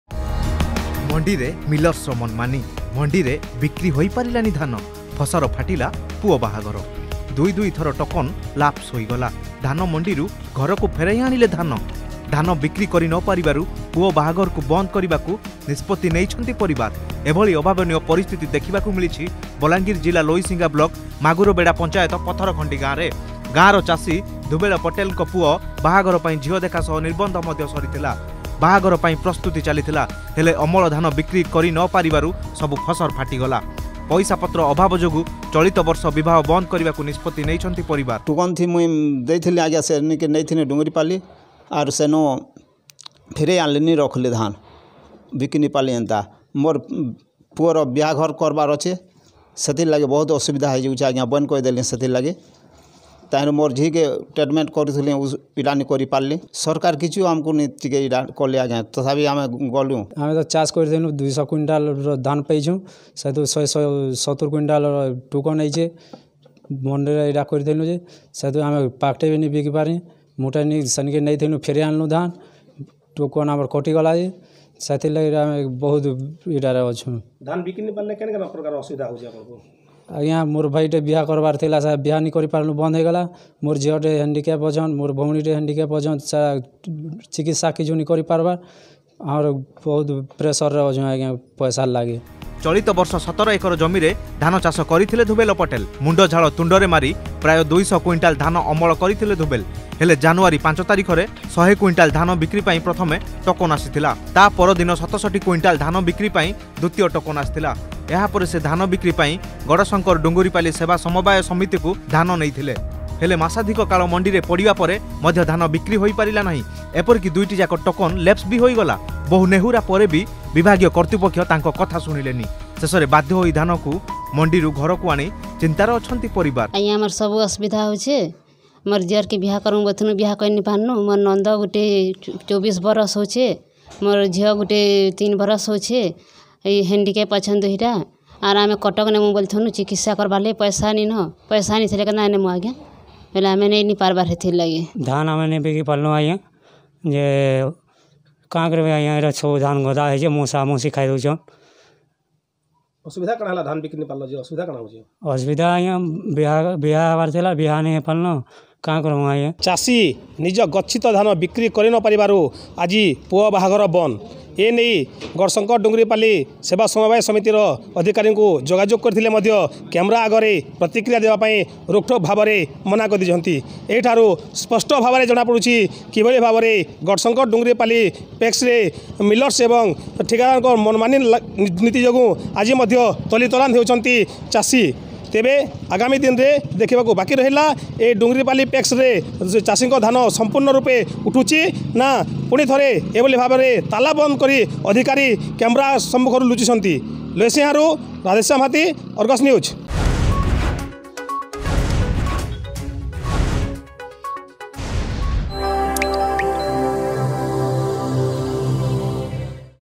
ମଣ୍ଡିରେ ମିଲର୍ସର ମନମାନି
ଭଲ ଧାନ ଫସଲ ଦେଖି ଚାଷୀ ଖୁସ ଥିଲା। ଧାନ ବିକ୍ରି ଗୁଜୁରାଣ ମେଣ୍ଟାଇବା ସହ ବିବାହ କାର୍ଯ୍ୟ କରିବା ପାଇଁ ସ୍ଥିର କରିଥିଲେ। ଏଥିପାଇଁ ନିର୍ବନ୍ଧ ମଧ୍ୟ ସରିଥିଲା। ହେଲେ ଅମଳ ଧାନ ବିକ୍ରି ନ କରିପାରିବାରୁ ପୁଅ ବିବାହ ବନ୍ଦ ହୋଇ ଯାଇଛି। ଏଭଳି ଅଭାବନୀୟ ପରିସ୍ଥିତି ଦେଖିବାକୁ ମିଳିଛି ବଲାଙ୍ଗିର ଜିଲ୍ଲା ଲୋଇସିଂହା ବ୍ଲକ୍ ପଥରଖଣ୍ଡି ଗାଁରେ...